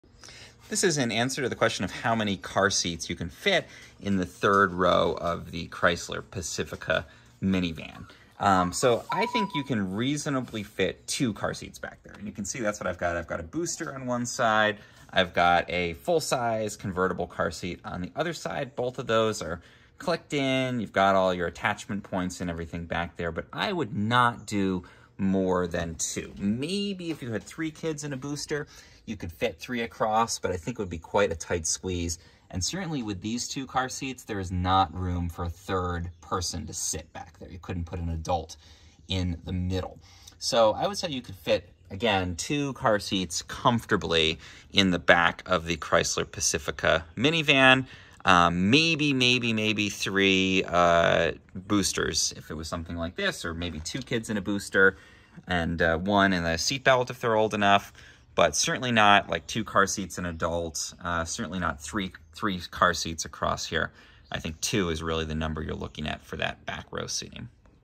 How Many Carseats Fit in Chrysler Pacifica 3rd Row?
I show how many carseats fit into the 3rd row of the Chrysler Pacifica minivan. The Hybrid 2022 model is shown. for full articles.

for
📱Hands-on reviews of the latest gadgets
💰Info on side hustles & niche websites
🛠️Exclusive discounts on creator tools